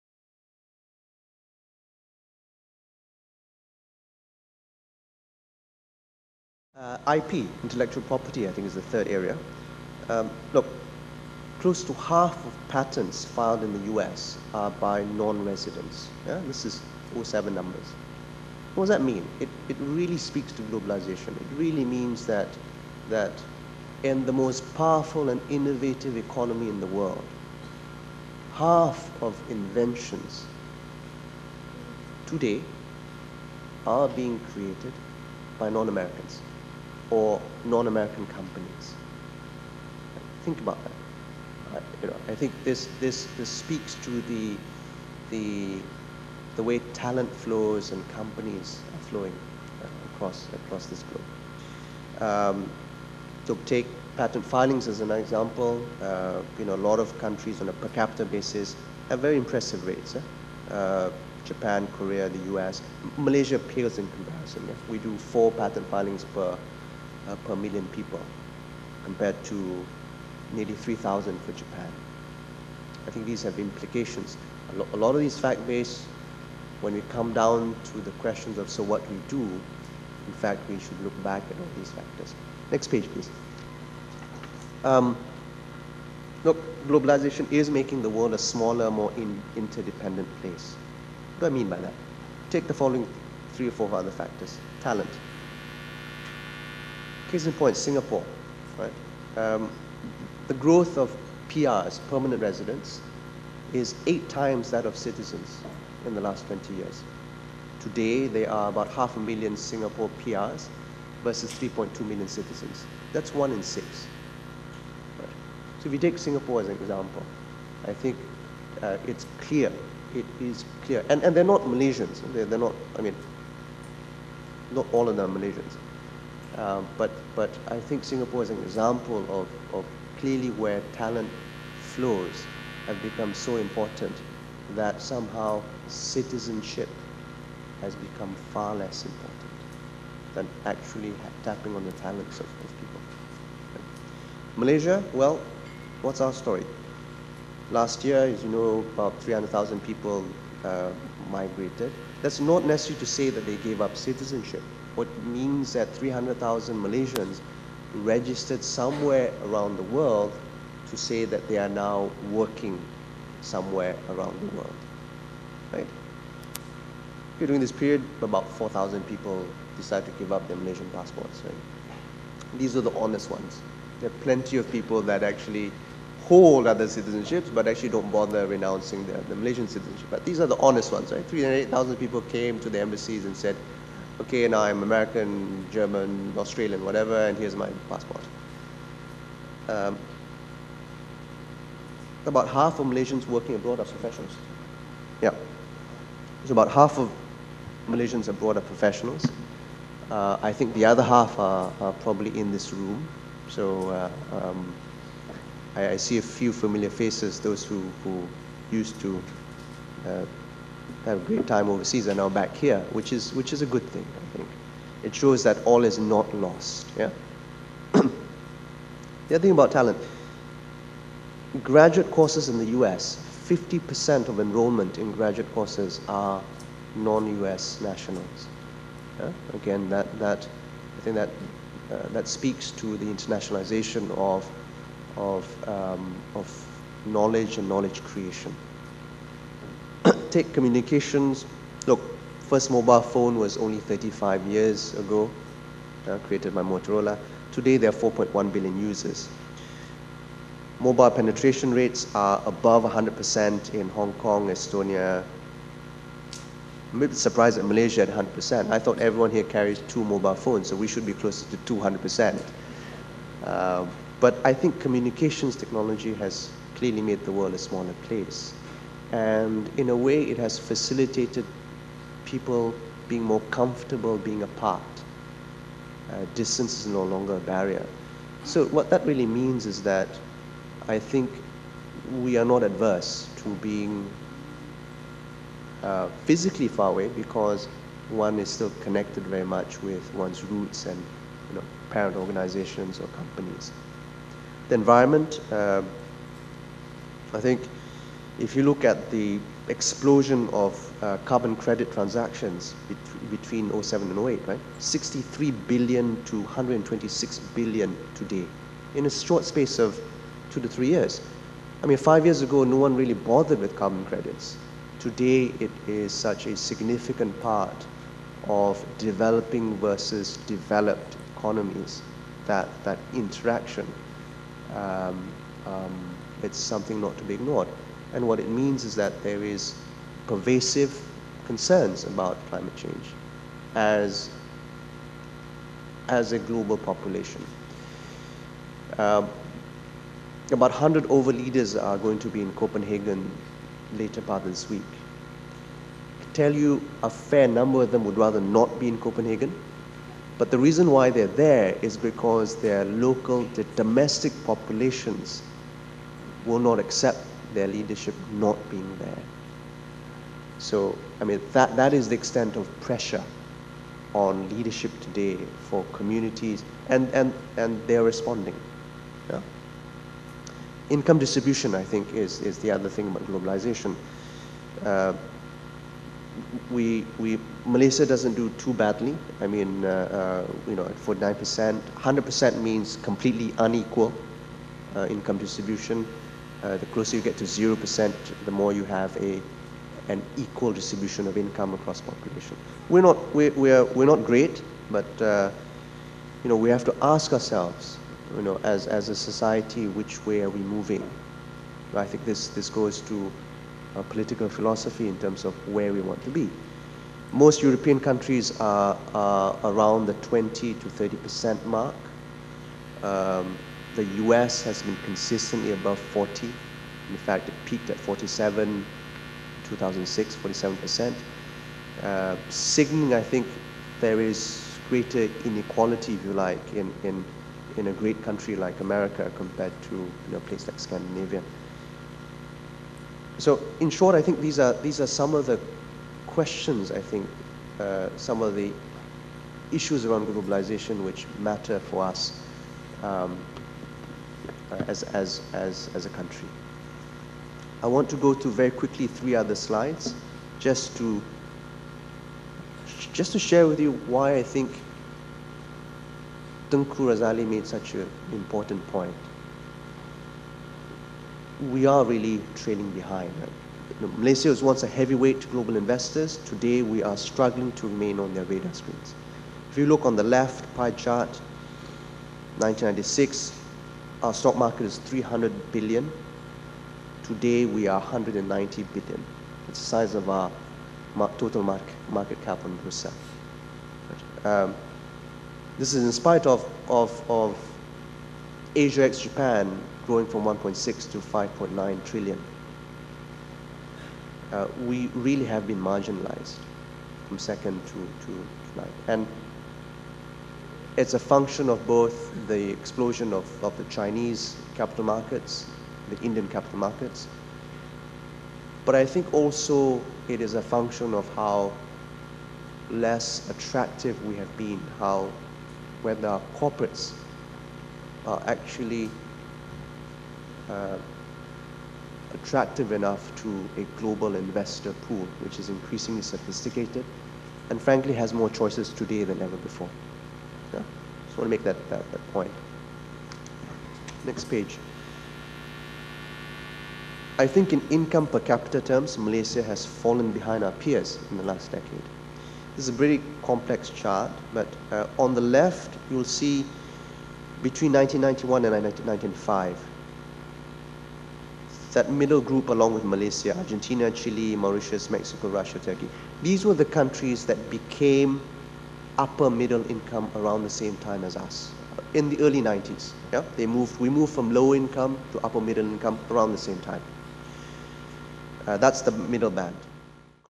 Young Corporate Malaysians 2009 - Omar Mustapha, Managing Principal & Director, Ethos & Company
Topic : Globalisation & Free Trade : Malaysia's Next Economic Strategy

Video shot and edited by Nigel Sia. to find out more.